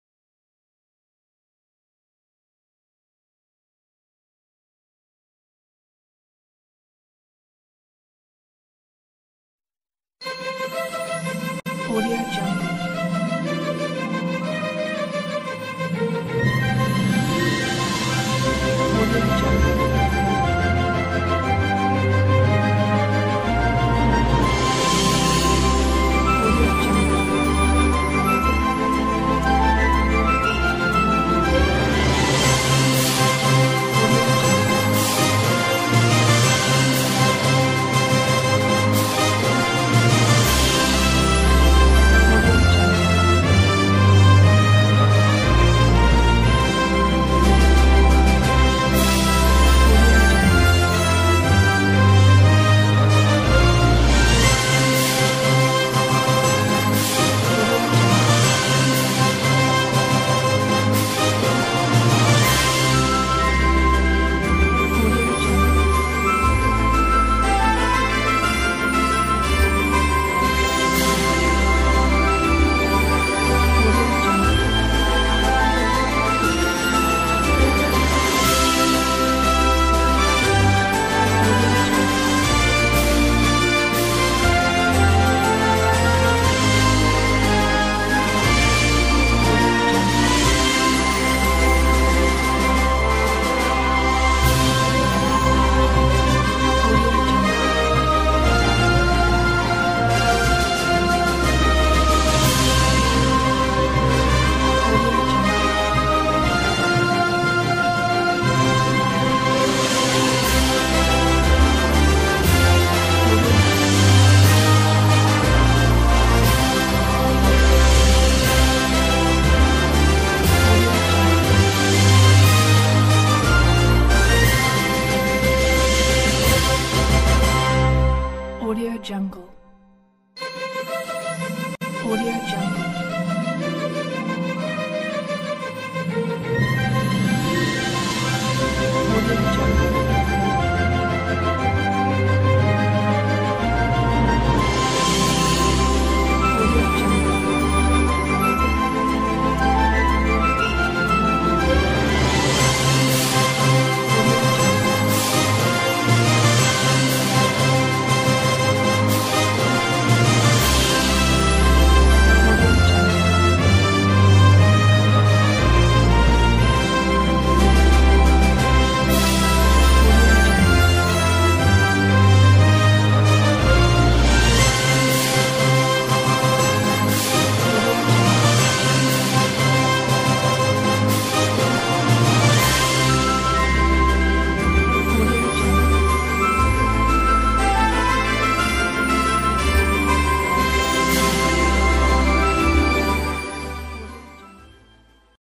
After Effects Project Files - Awards Show Package II - VideoHive 7713839
MAIN FUTURES: After Effects CS4 , CS5 , CS5 .5 , CS6 Compatible Opener Slideshow Info Box Bumper Nominations Best award categories Award Winner Video Box Lower third End Credits Full HD 1920x... (broadcast-packages) Read more